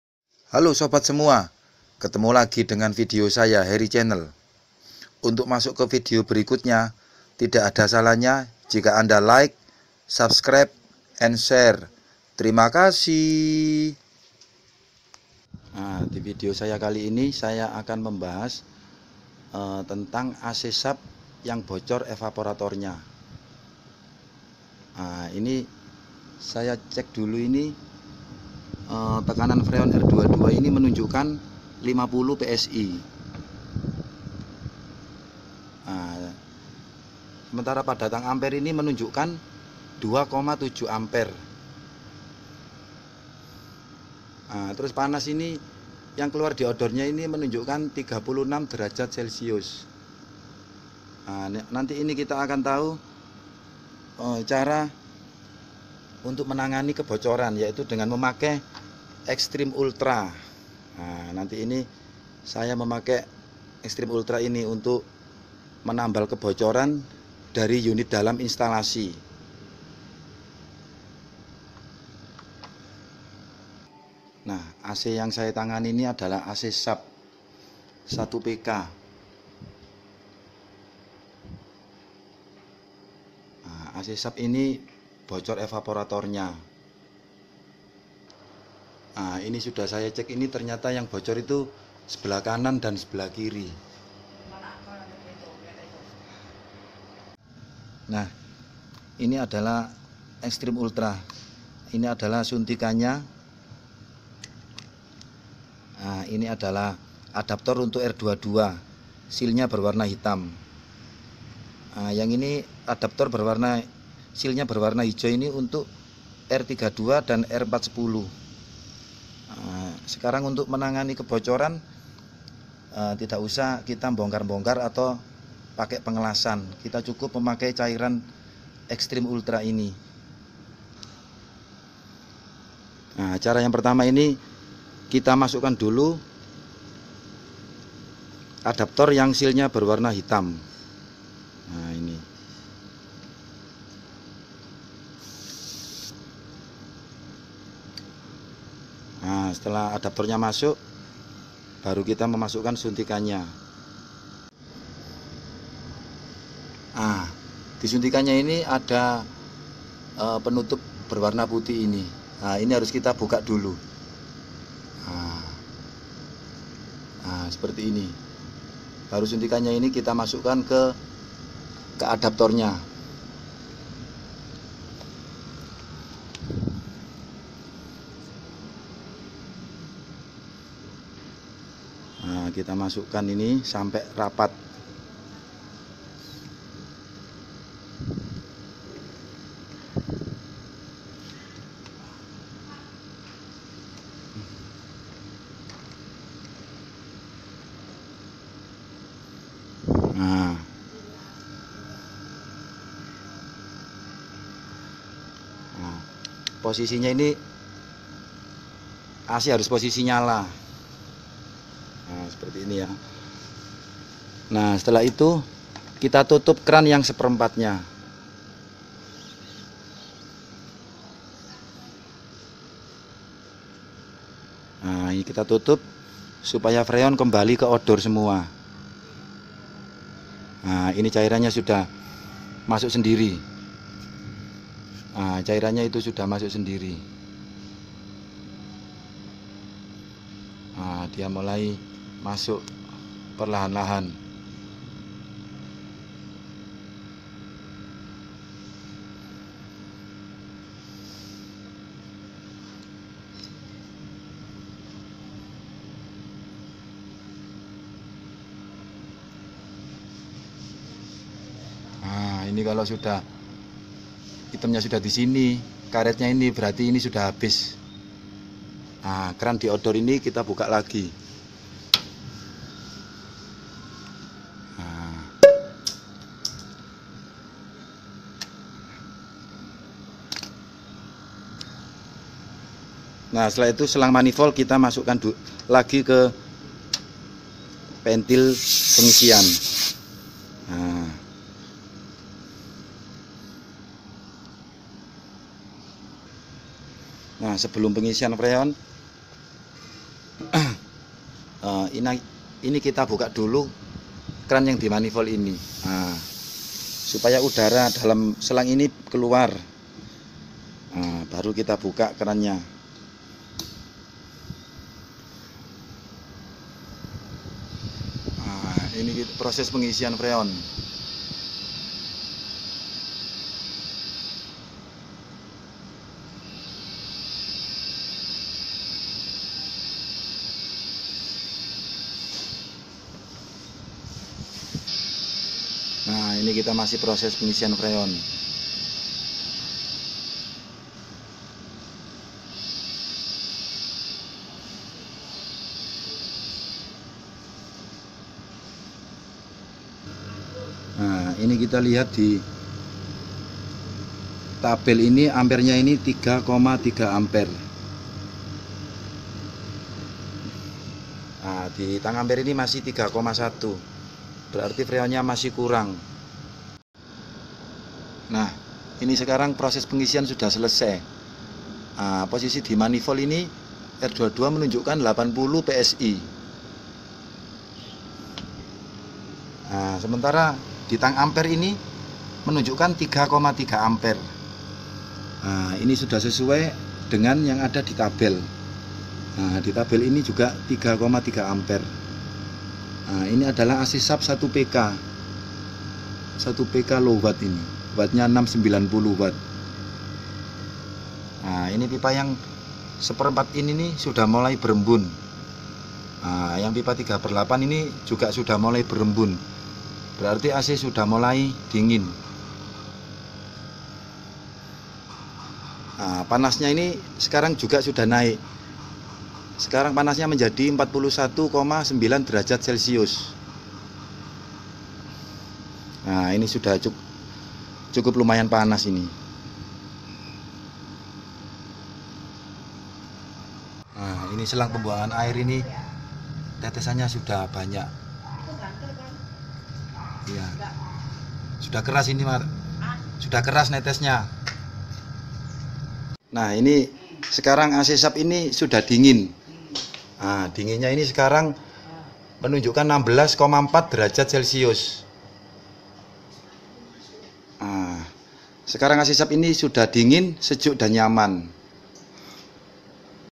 Karena ini perbaikan evaporator ac yang bocor sekarang lebih gampang dan mudah.
Ternyata perbaikan evaporator ac yang bocor sekarang gampang dan mudah, tidak usah bongkar ac, tidak usah memakai pengelasan.

Dengan menggunakan cairan Liquid penambal kebocoran dari unit dalam instalasi, sekarang lebih cepat dan praktis.

TERIMA KASIH SUDAH MELIHAT VIDEO SAYA.

Salam SDN : Sejuk, Dingin dan Nyaman...